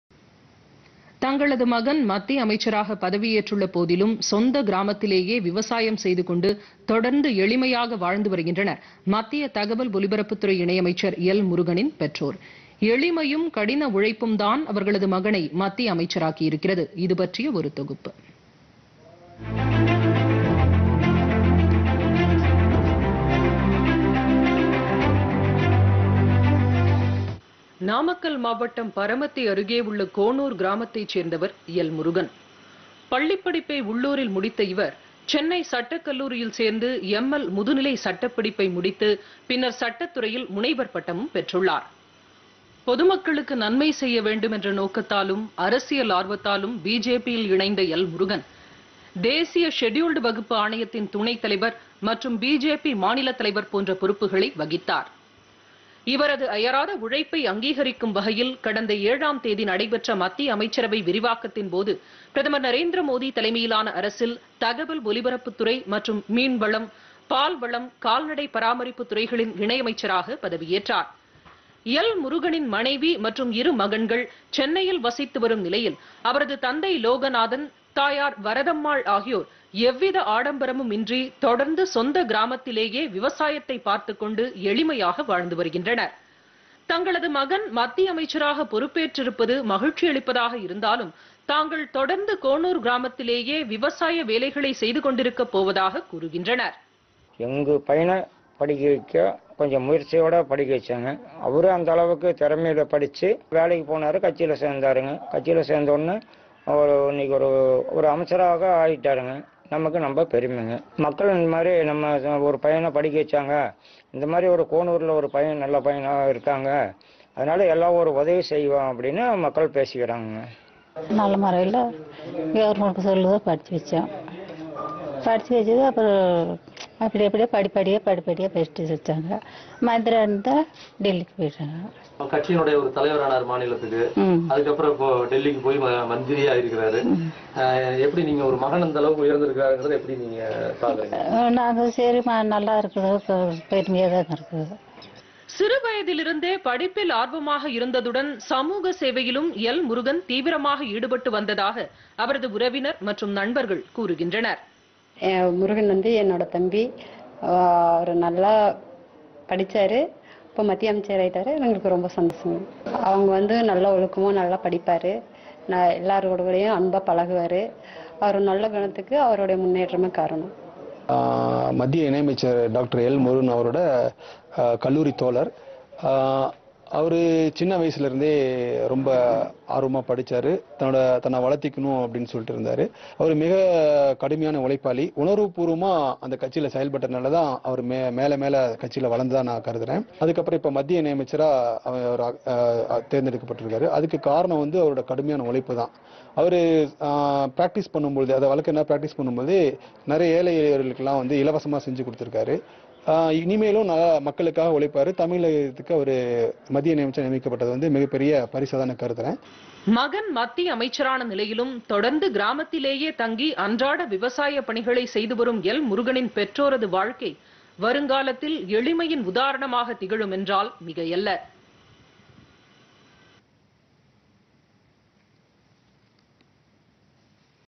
எளிமையும், கடின உழைப்பும்
தங்களது மகன் மத்திய அமைச்சராக பதவியேற்றுள்ள போதிலும், சொந்த கிராமத்திலேயே விவசாயம் செய்துகொண்டு, தொடர்ந்து எளிமையாக வாழ்ந்து வருகின்றனர், மத்திய தகவல் ஒலிபரப்புத்துறை இணையமைச்சர் எல்.முருகனின் பெற்றோர். எளிமையும், கடின உழைப்பும்தான்  அவர்களது மகனை மத்திய அமைச்சராக்கியிருக்கிறது.